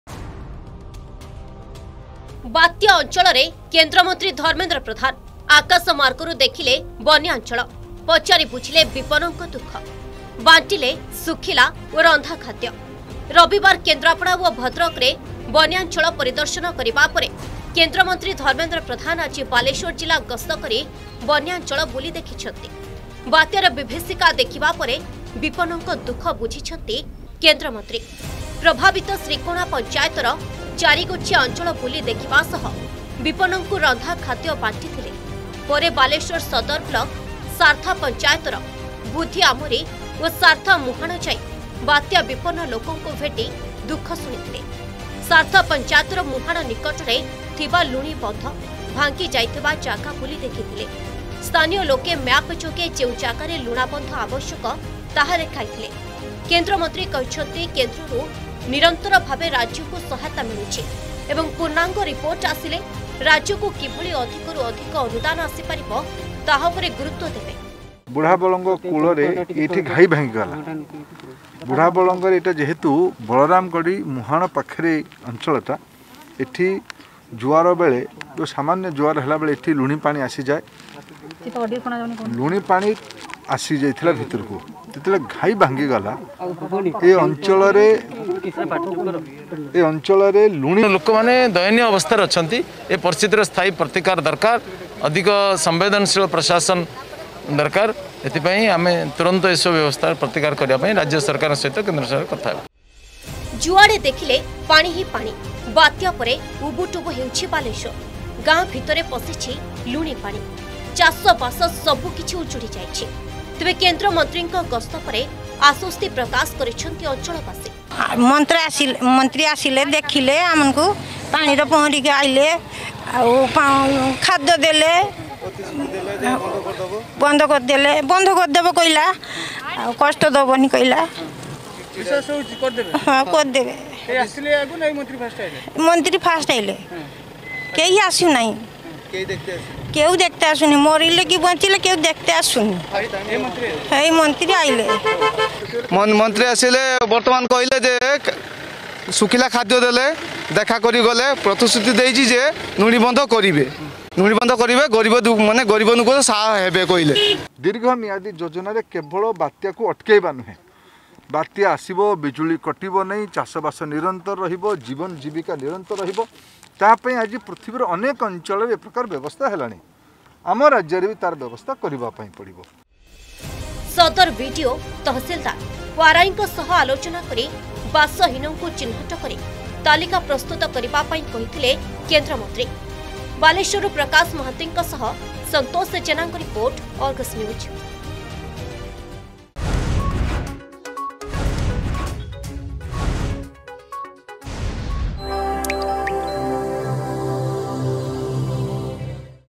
ବାତ୍ୟାଞ୍ଚଳରେ କେନ୍ଦ୍ରମନ୍ତ୍ରୀ ଧର୍ମେନ୍ଦ୍ର ପ୍ରଧାନ
ବାଲେଶ୍ବରରେ ବାତ୍ୟାଞ୍ଚଳ ପରିଦର୍ଶନ କଲେ କେନ୍ଦ୍ରମନ୍ତ୍ରୀ ଧର୍ମେନ୍ଦ୍ର ପ୍ରଧାନ। ଆକାଶ ମାର୍ଗରୁ ବାତ୍ୟାଞ୍ଚଳ ବୁଲି ଦେଖିବା ସହ ବିପନ୍ନଙ୍କ ଦୁଃଖ ବୁଝିଛନ୍ତି